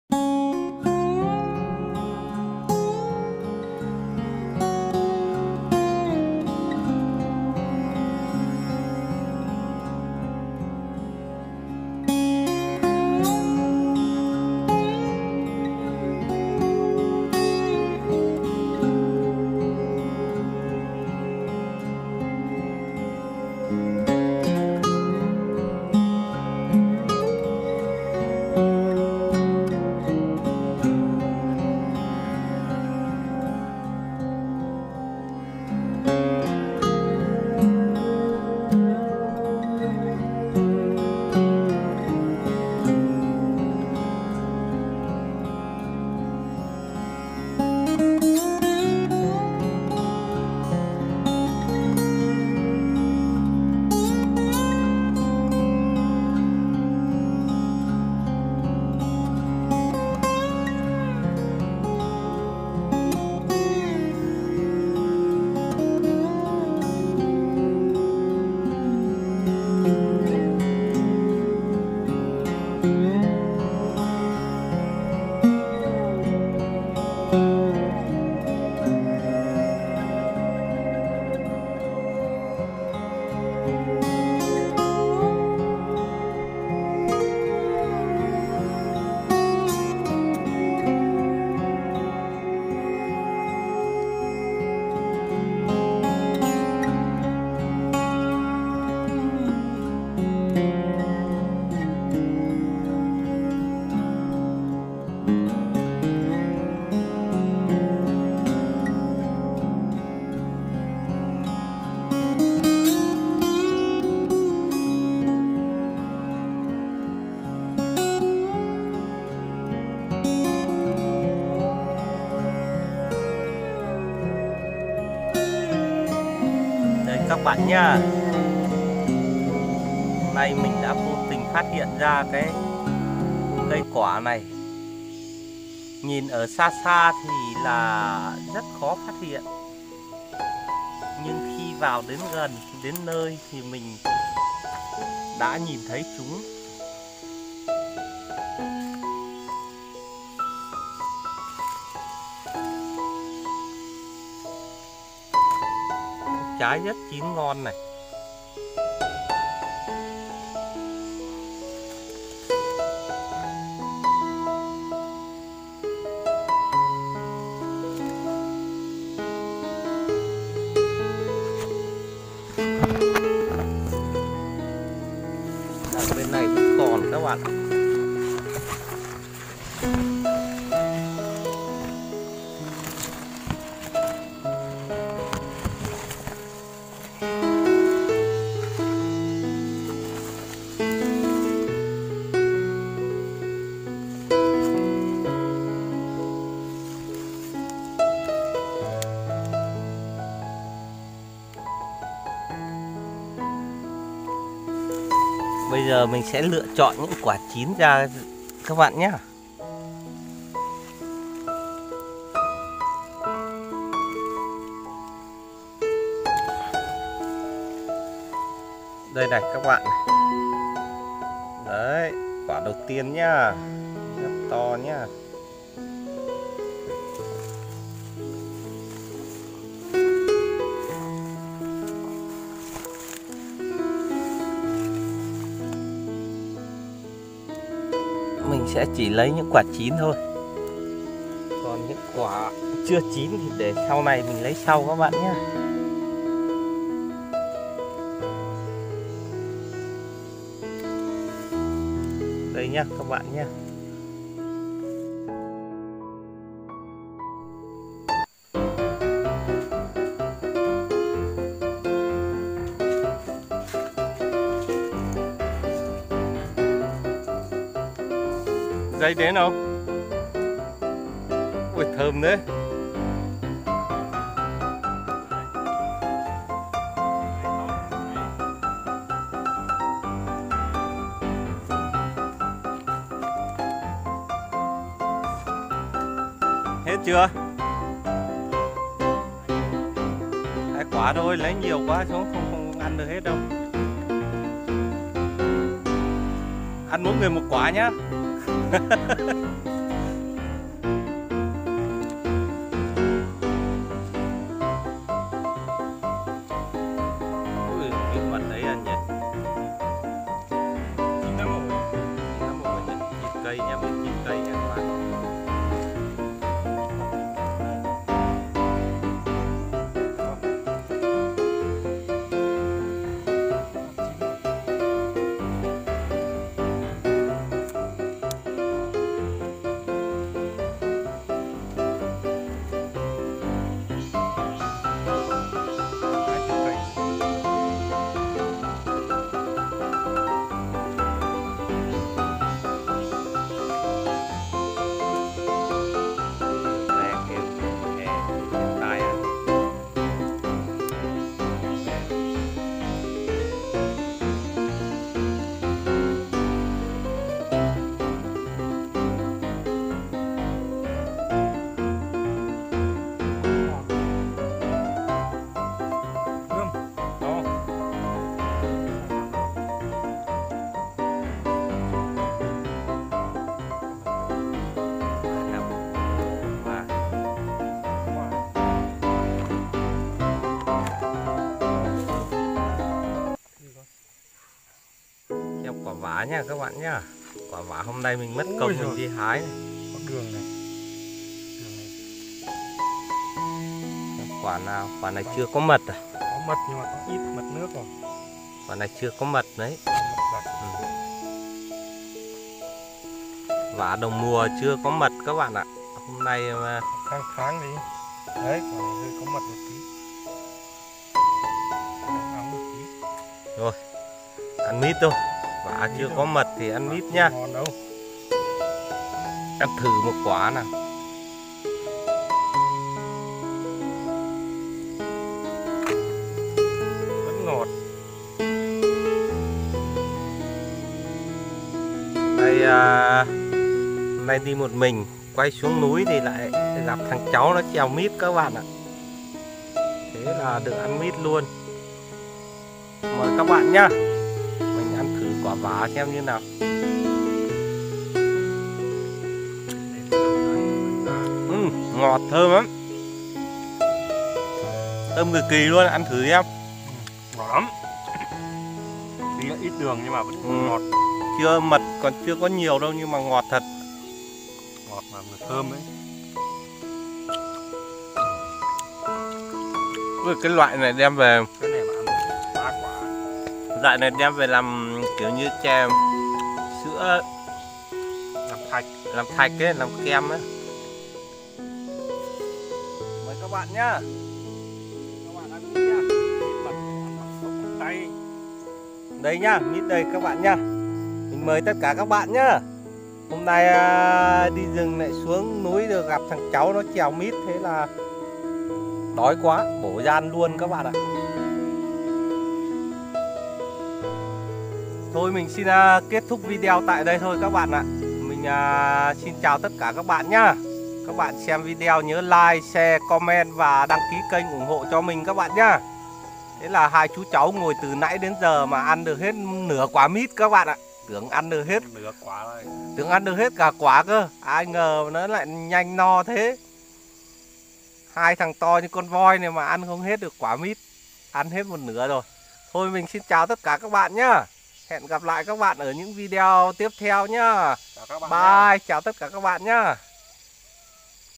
Trái này ăn rất ngon. Tam dinh vlog
đi tìm trái cây mọc tự nhiên, vô tình gặp trái này ăn rất ngon.